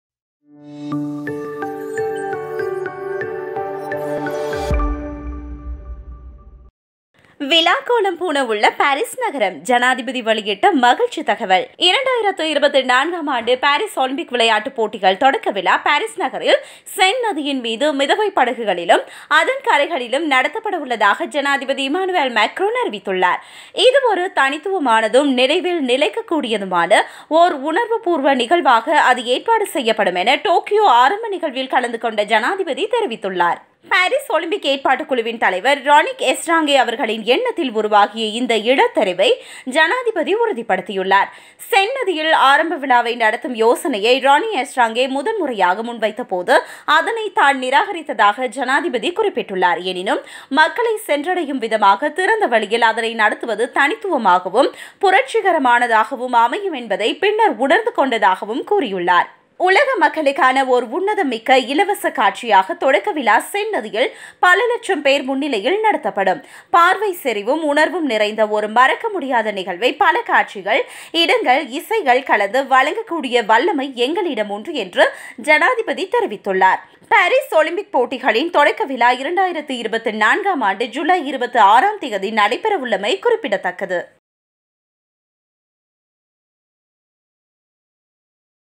விழாக் கோலம் பூணவுள்ள பாரிஸ் நகரம்! ஜனாதிபதி வெளியிட்ட மகிழ்ச்சித் தகவல்
2024 ஆம் ஆண்டு பாரிஸ் ஒலிம்பிக் விளையாட்டுப் போட்டிகளின் தொடக்க விழா பாரிஸ் நகரில் சென் நதியின் மீது மிதவைப் படகுகளிலும் அதன் கரைகளிலும் நடத்தப்படவுள்ளதாக ஜனாதிபதி இமானுவெல் மக்ரோன் அறிவித்துள்ளார்.